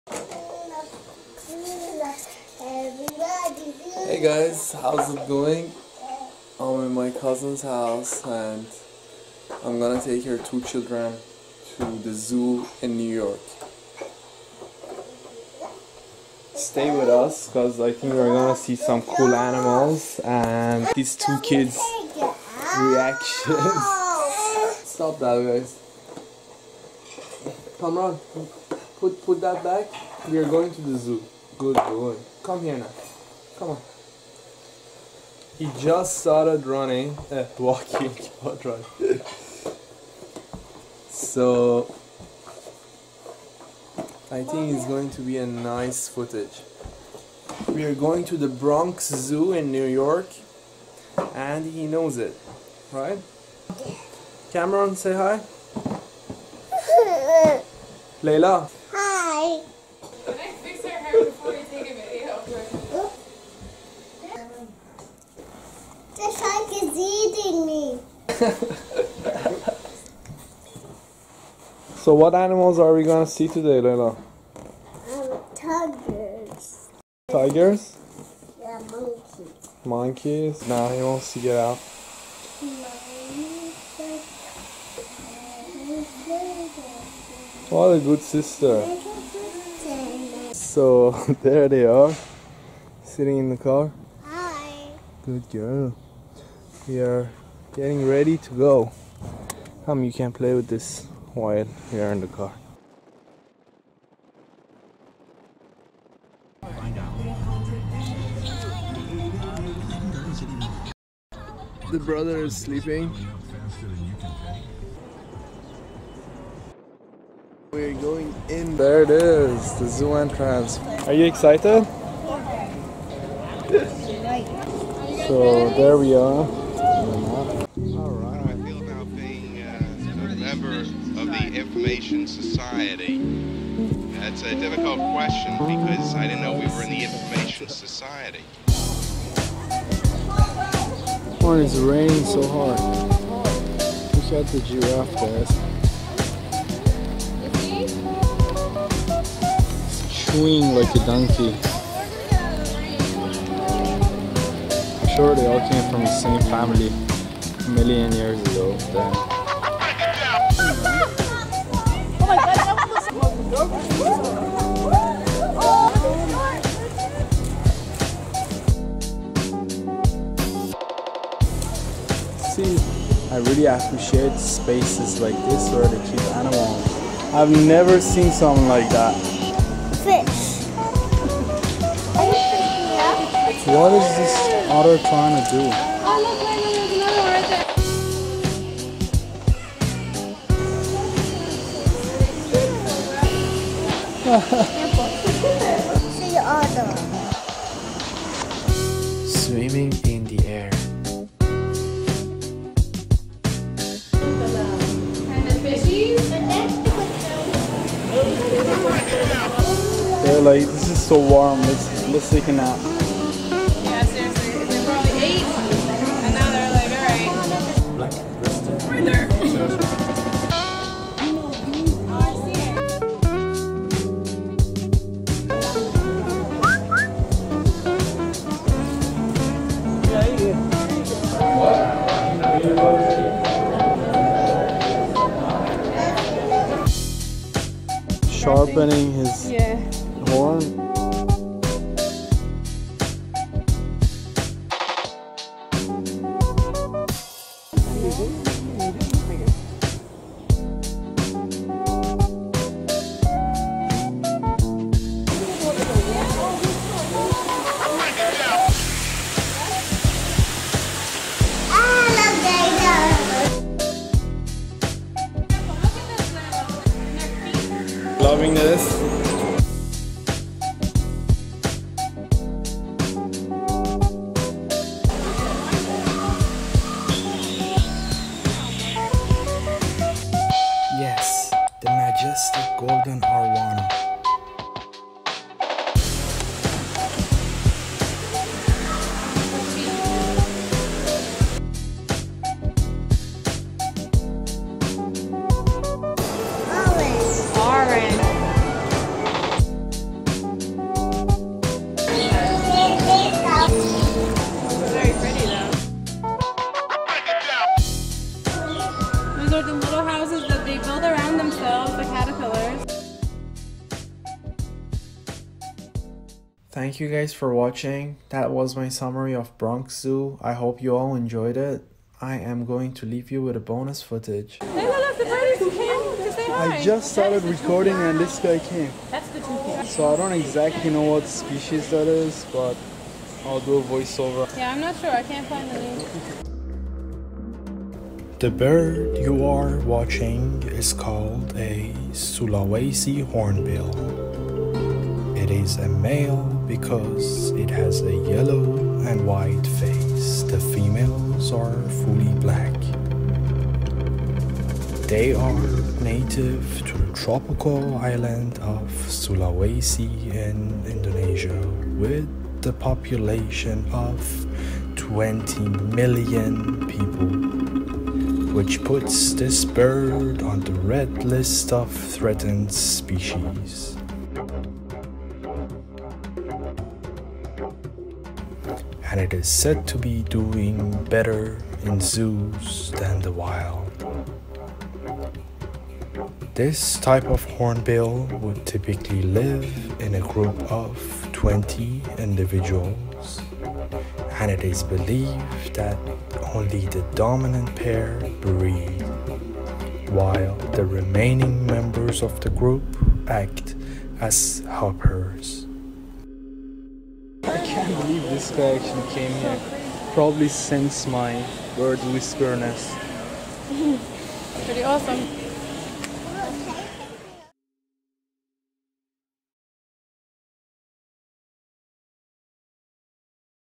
New York City's Bronx Zoo adventures with the Kids!
I took this  video a while back when I was visiting family in the East coast. My cousin and I decided to take her kids to the zoo since we all love animals and so that I could make a video for you guys. Bronx Zoo is huge and I probably covered only one third of their animals; since it was raining we were indoor most of the time specifically at the heated tropical rain forest section.